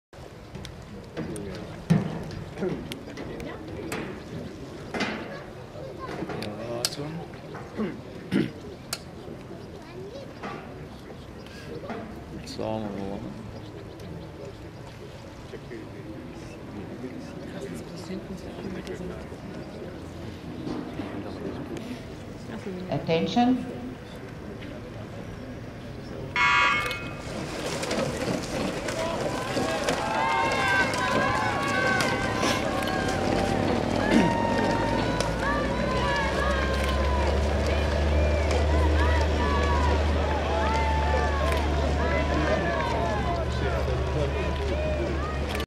Kathryn Ross and John Maclean heat row.wmv
The start of the heat row for Kathryn Ross and John Maclean at the 2011 FISA World Rowing Championships in Bled, Slovenia.